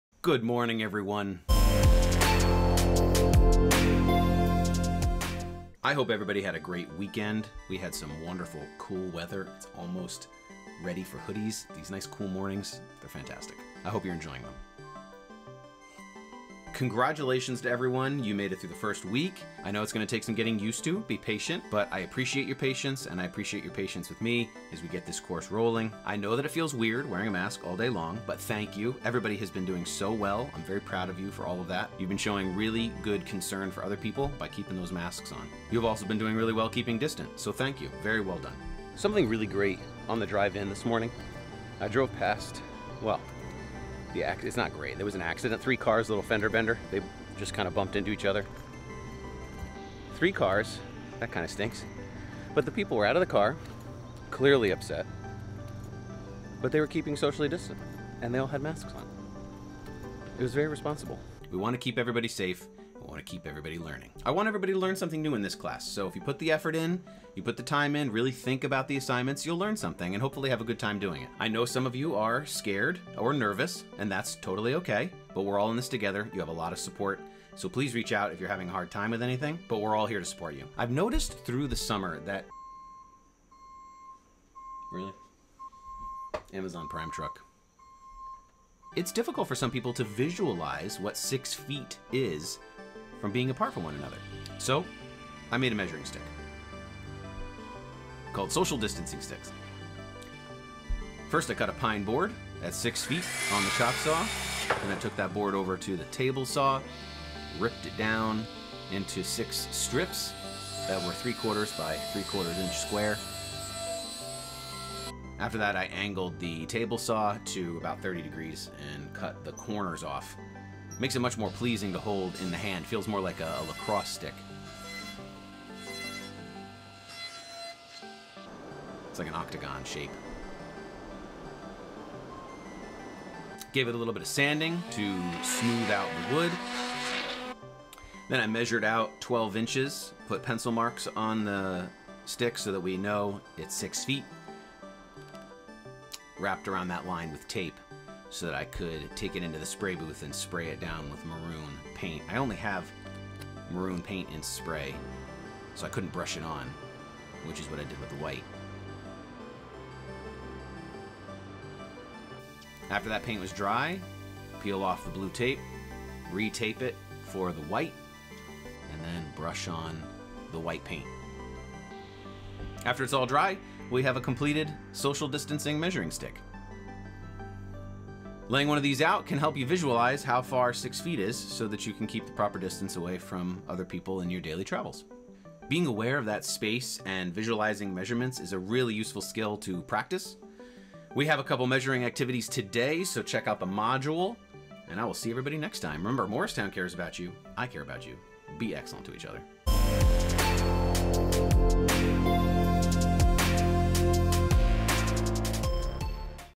Social Distancing Sticks! VLOG #47
Good Morning! Sept. 14,15, 2020
This video is a part of a series that I produced for students in my Woods classes during the 2020/2021 school year.